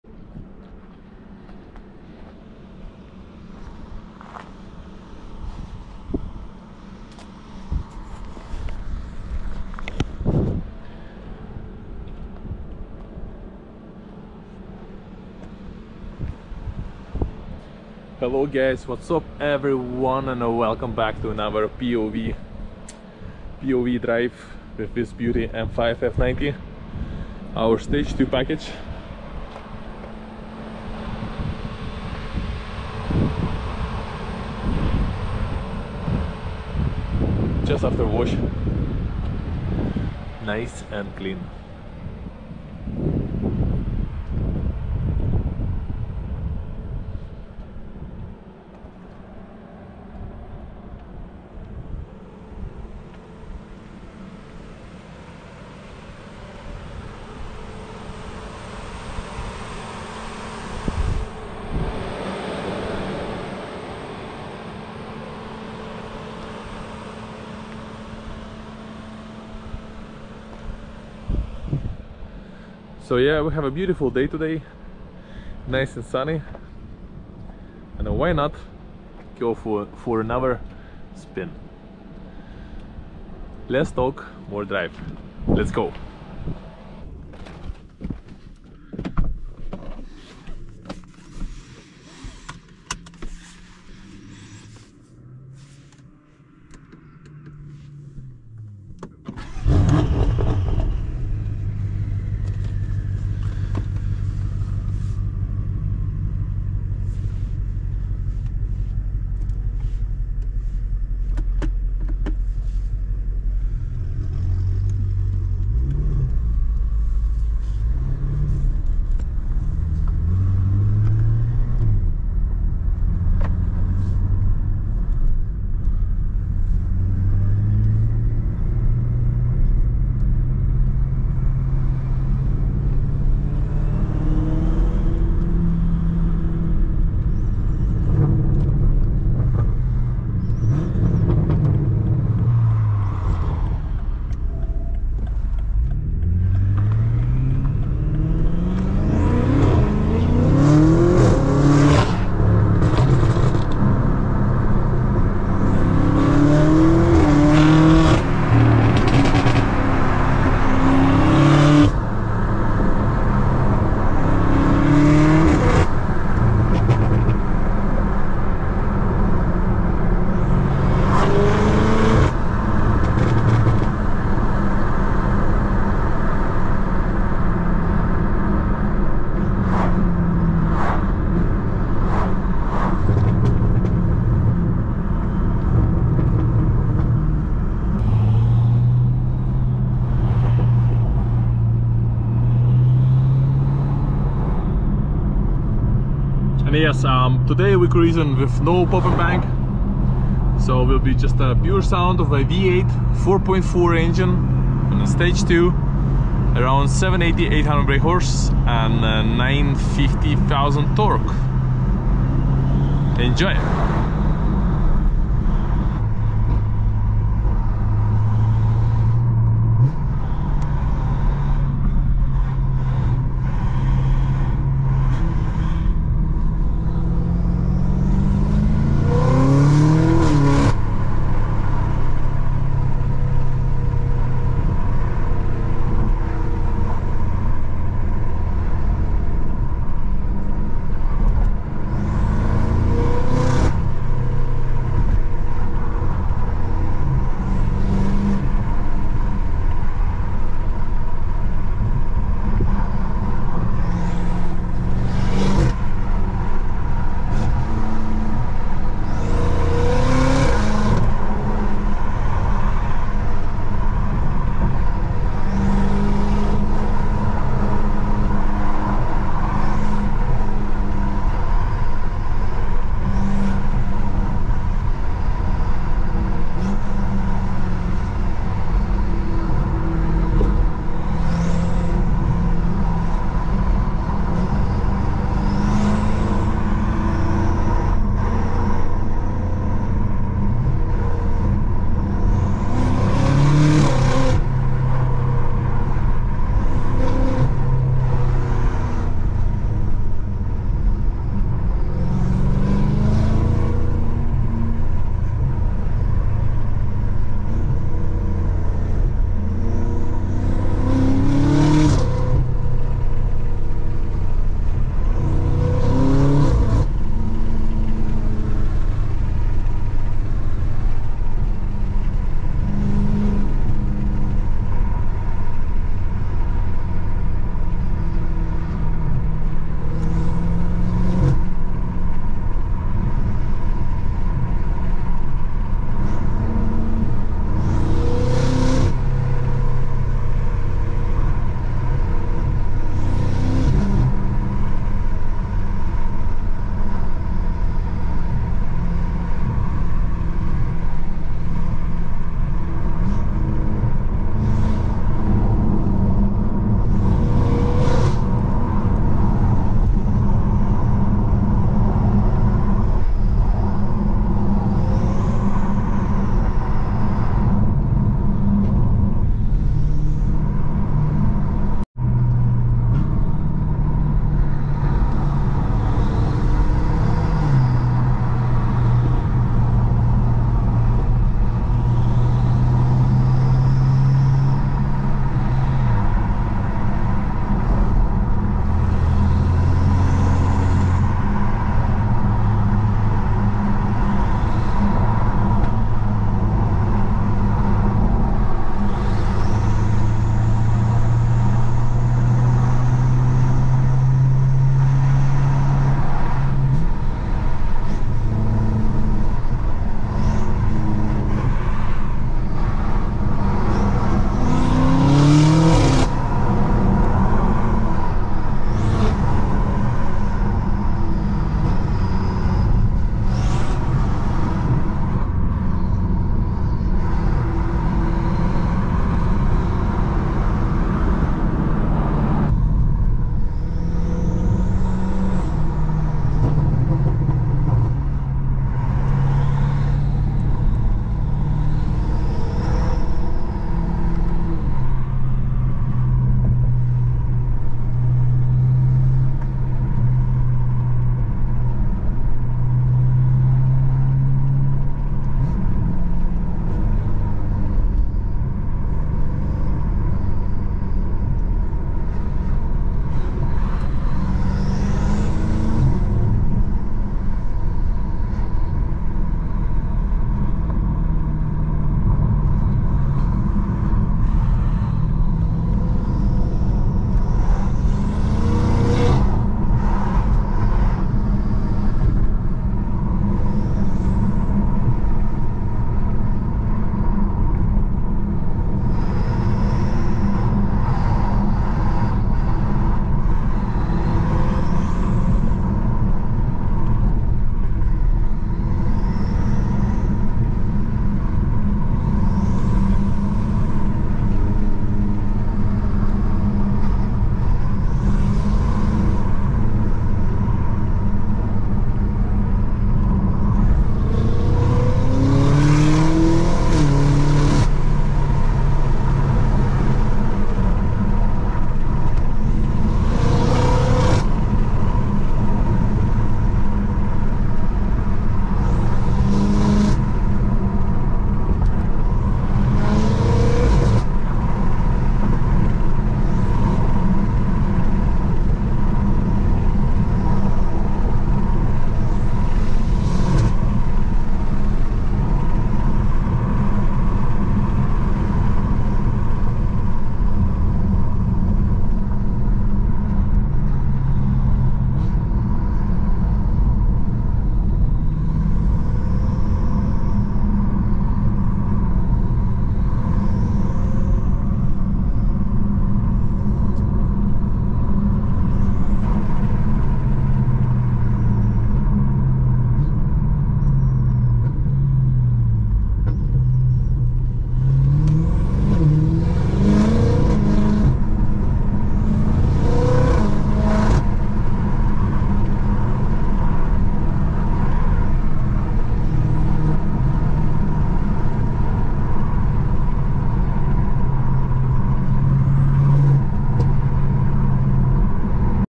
POV M5 F90 v8 engine sound.
Just a quick pov drive on m5 f90. Day drive with new LED custom build steering wheel, some REVS no pop and bangs.